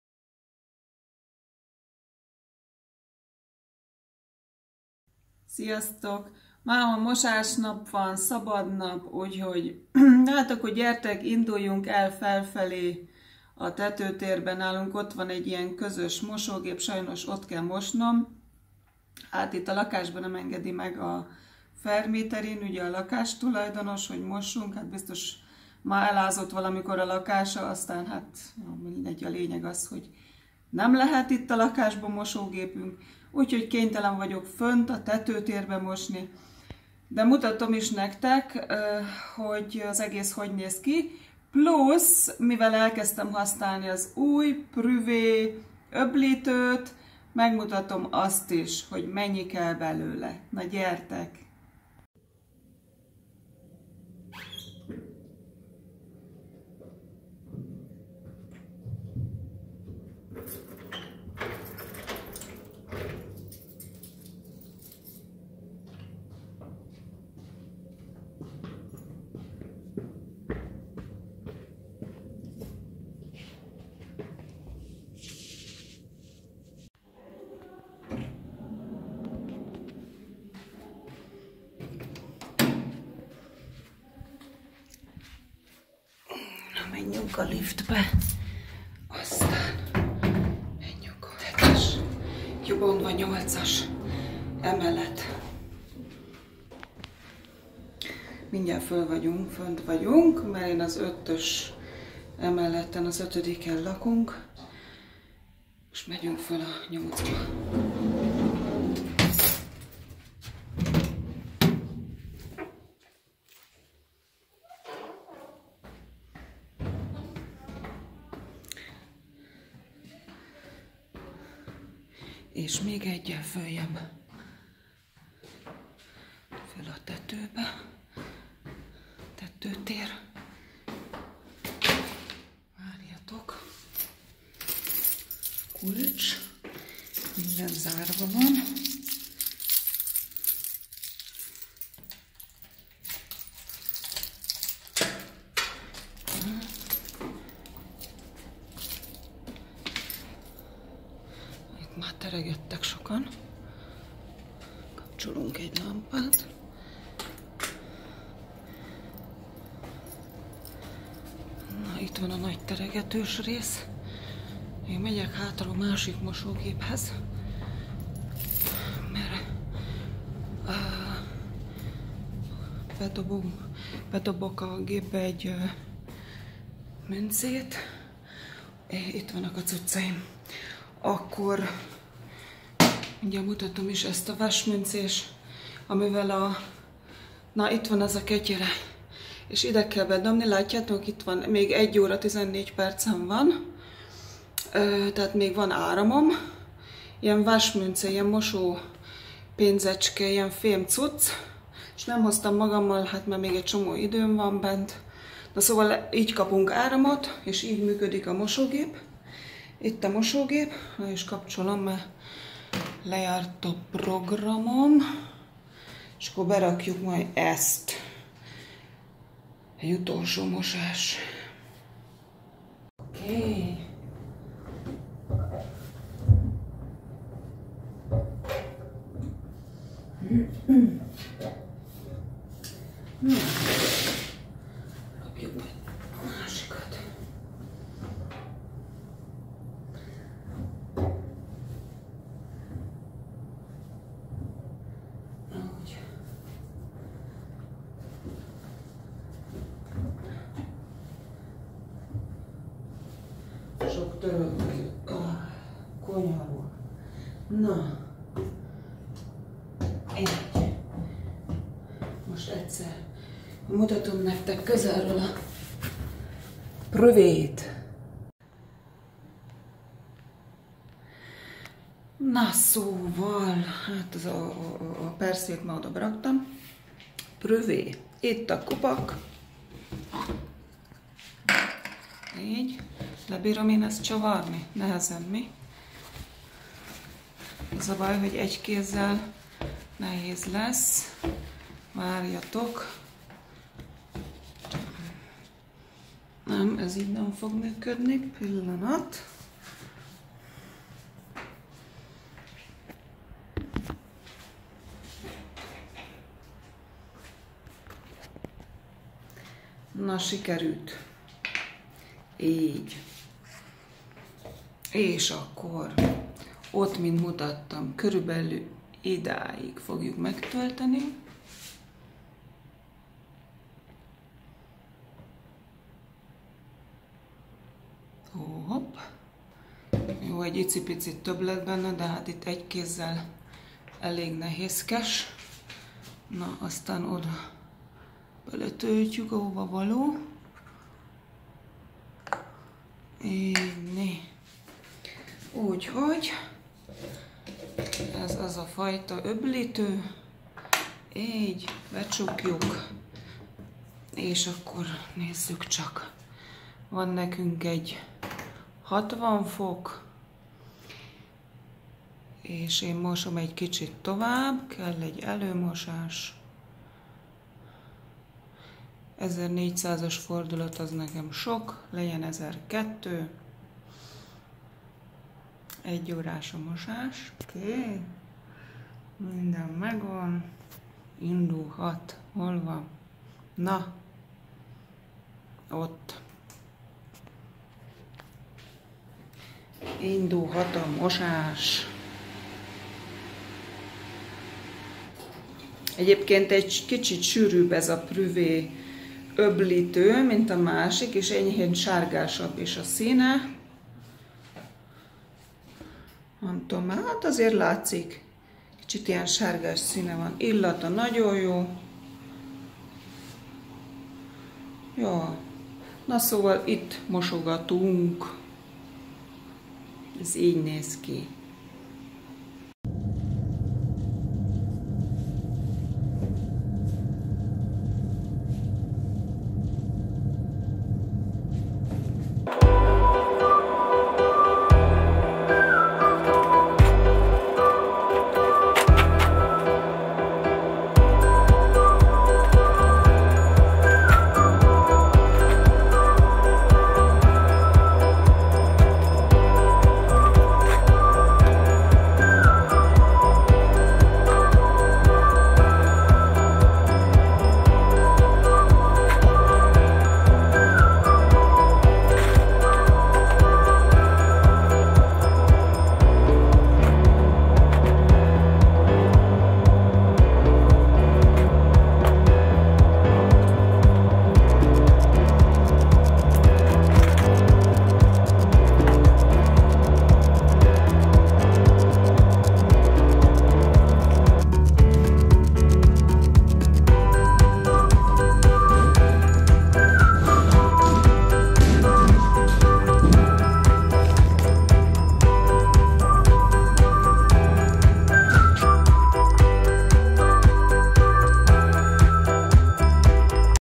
Mosásnap - közös mosógép a tetőtérben, Németország
Közös mosógép a lakókkal? Németországban mi ilyen helyen lakunk. A videóból kiderül hogy miért van ez így.